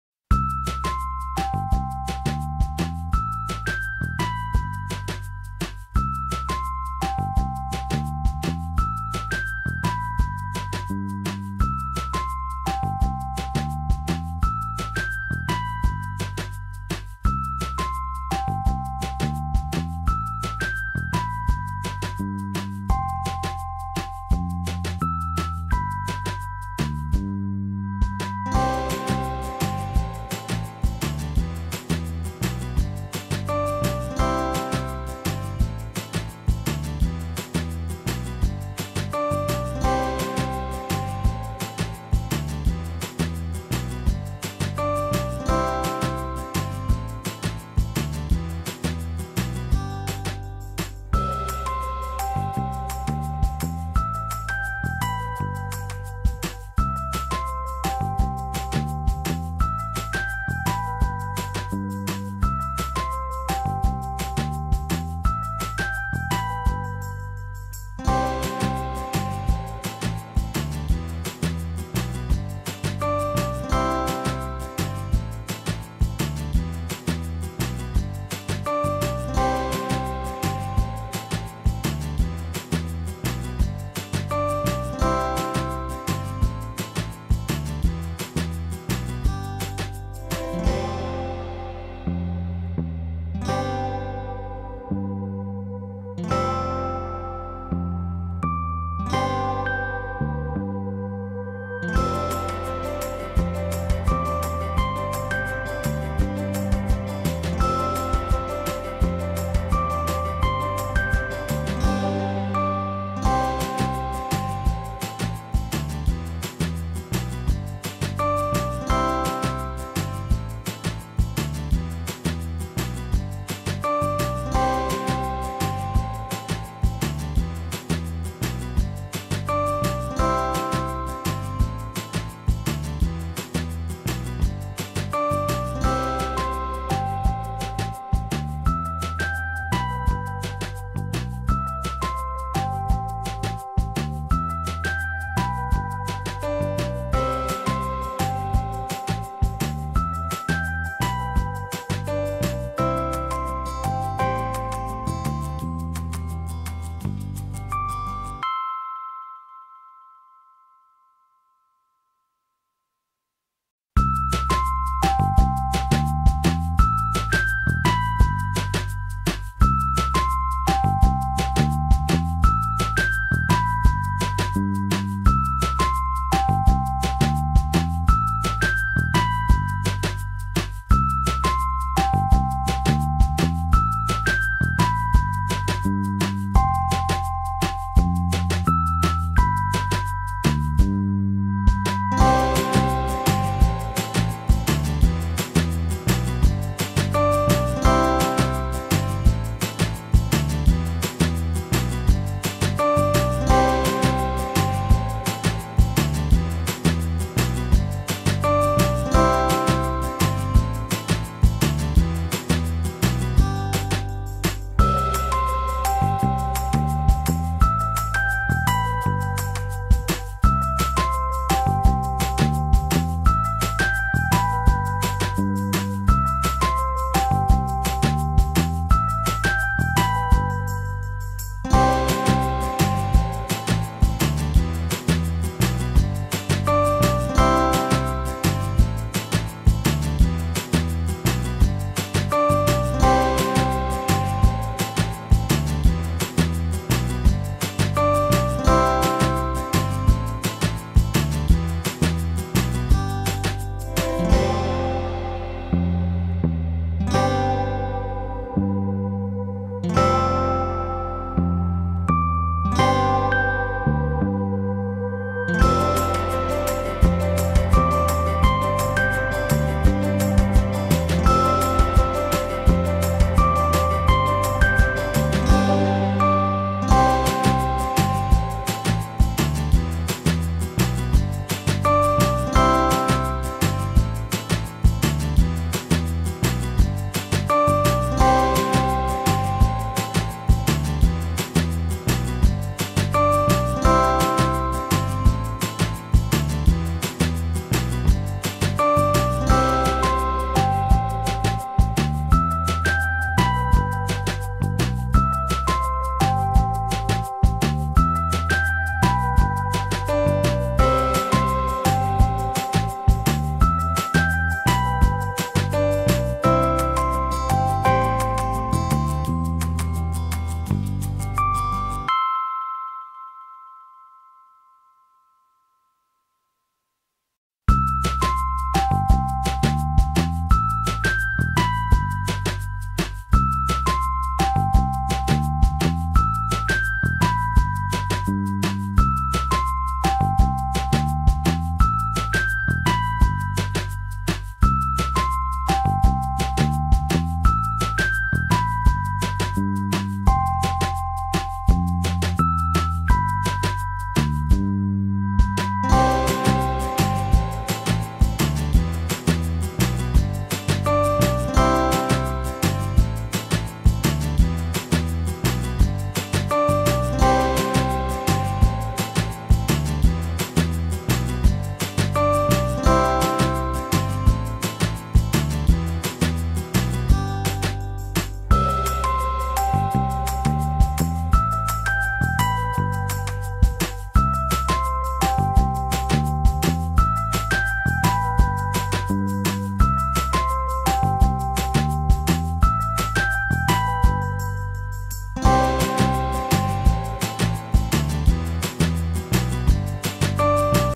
Бантики Канзаши из лент 2,5 см. Мастер Класс / Bows Kanzashi ribbon 2.5 cm Master Class
Бантики Канзаши из репсовых  лент и кружева 2,5 см. Мастер Класс

Bows Kanzashi from REP ribbons and lace 2.5 cm Master Class

Fitas Канзаши de репсовых de fitas e rendas 2,5 см. o Assistente de Classe